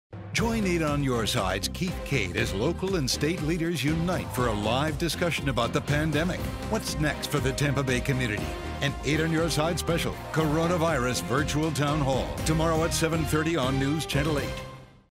WFLA 8 Virtual Town Hall Meeting
Local and state leaders unite for a live discussion about the pandemic. WFLA News Channel 8's Coronavirus Virtual Town Hall meeting.